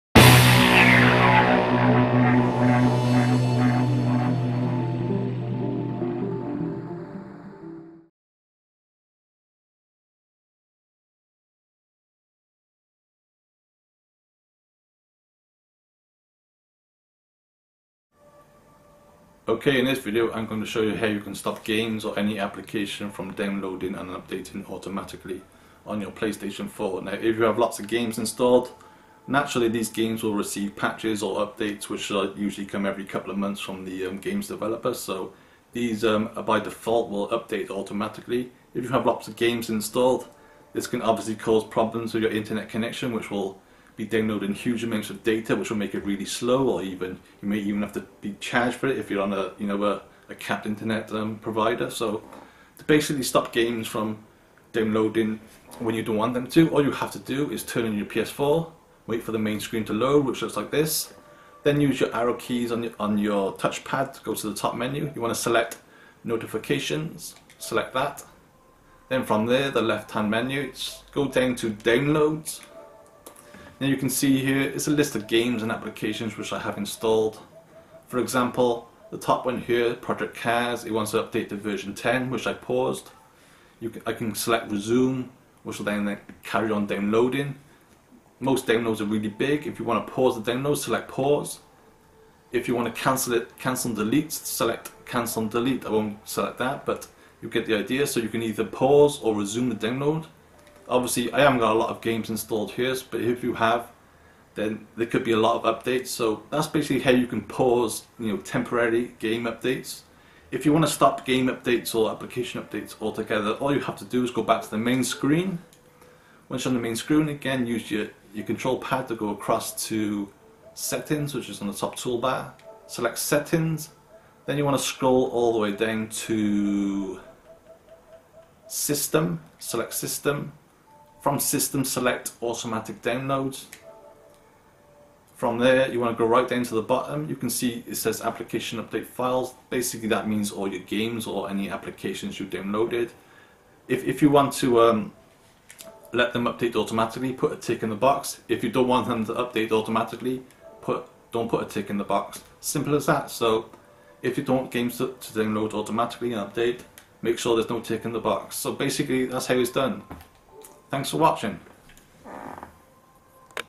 How to Stop Your PS4 from Downloading and Updating Games Automatically
In this video guide, I will show you how you can stop your PlayStation 4 from automatically downloading and updating games in the background. When you have lots of games installed, these will naturally receive updates from the games developers, which sometimes can be over 10 GB in size. This can cause your Internet connection to run really slowly and therefore it's a good idea to either pause or cancel them altogether.